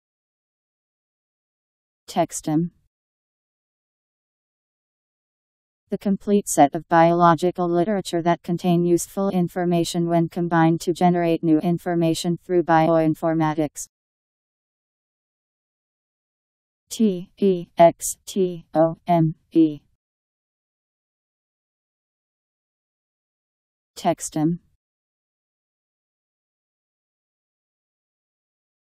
Textome Meaning
Video shows what textome means. The complete set of biological literature that contain useful information when combined to generate new information through bioinformatics..  Textome Meaning. How to pronounce, definition audio dictionary. How to say textome. Powered by MaryTTS, Wiktionary